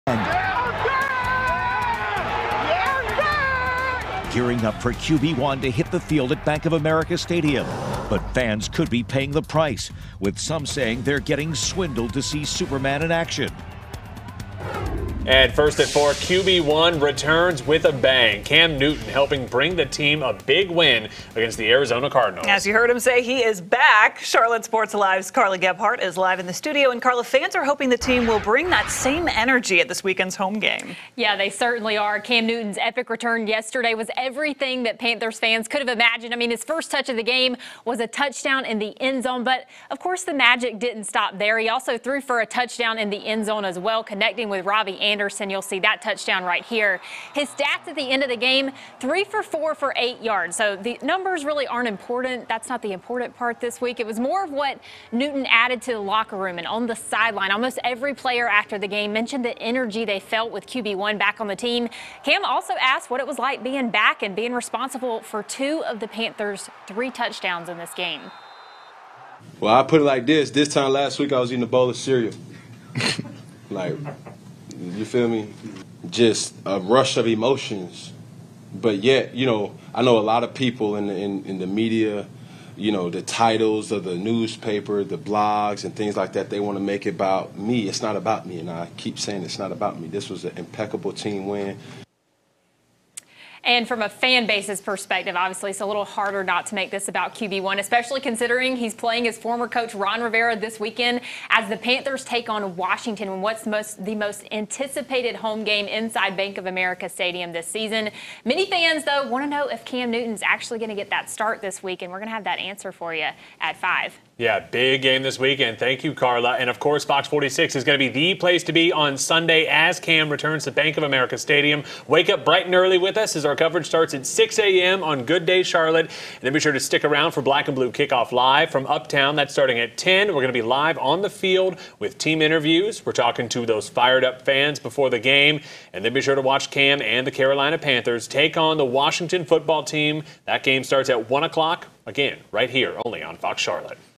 QB1 Cam Newston returning with a bang!
Cam Newton helping bring the team to a win against the Cardinals over the weekend! Fans are hoping the team will bring the same energy at this weekend's upcoming home game against the Washington Football Team.